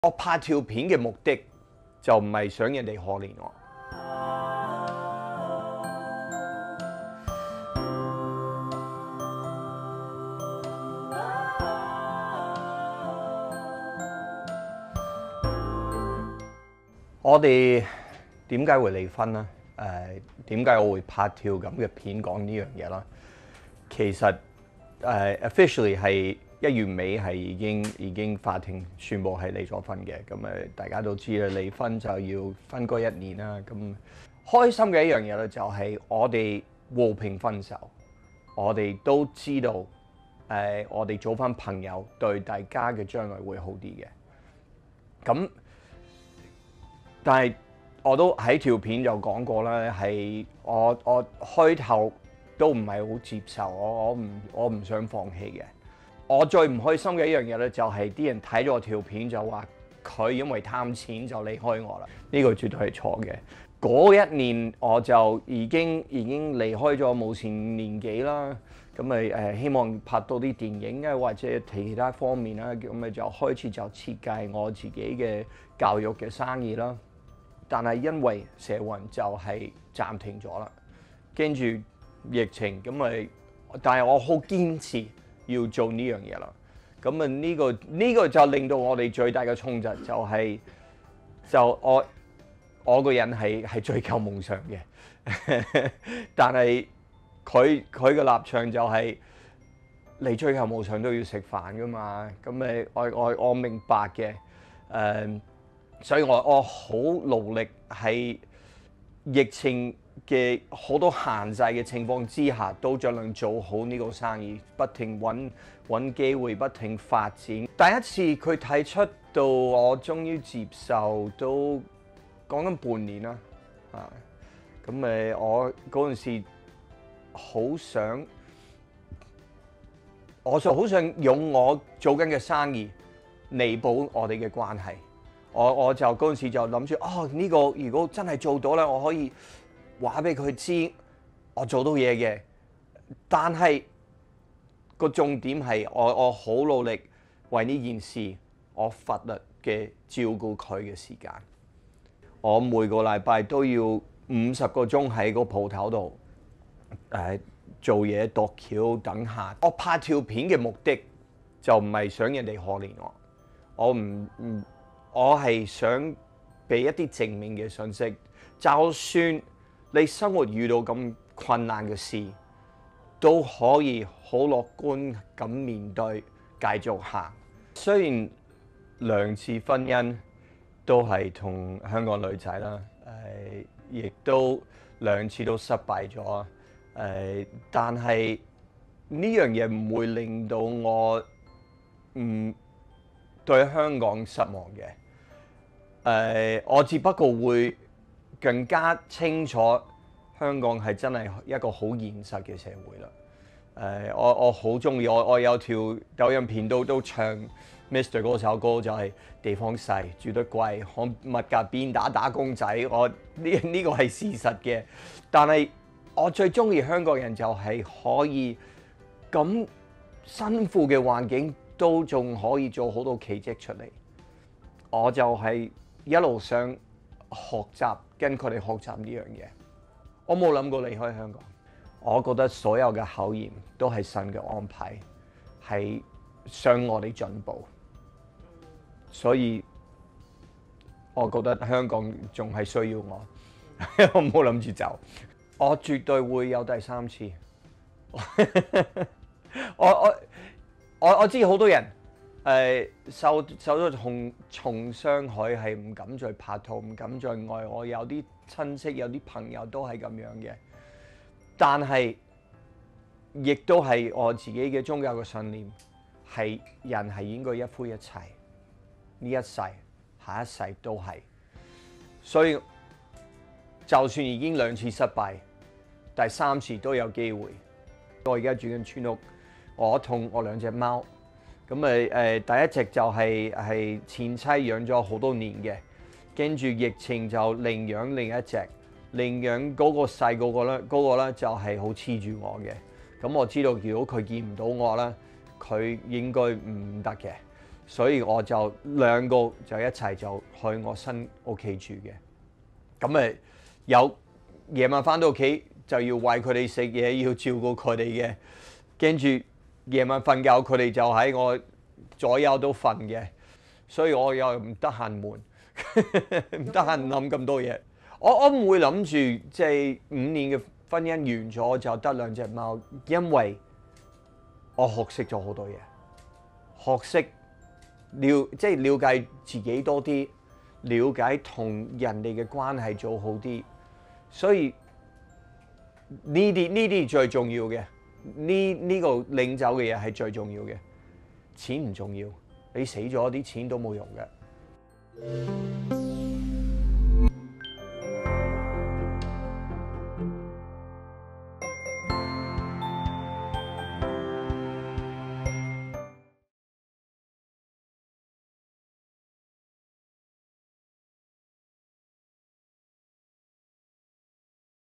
結束五年婚姻只剩兩隻貓 布偉傑「不想別人可憐我」│布偉傑專訪
娛樂圈御用外國人 布偉傑（阿一）早前在社交網站宣布結束第二段 婚姻，片中透露受疫情影響演出少了，加上其教育事業又不似預期，令他收入大減，夫妻生活模式變了，被前妻提出 離婚；離婚後的他帶着兩隻愛 貓搬到新界八鄉的村屋展開新生活，他澄清前妻不是拜金女，拍片公開離婚亦不是要別人 可憐，他是想發出正面訊息，即使你生活遇到那麼大的困難，都可以樂觀面對，積極前行。

1:00 前妻非貪錢
2:40 不想別人可憐
4:40 跟兩隻貓展開新生活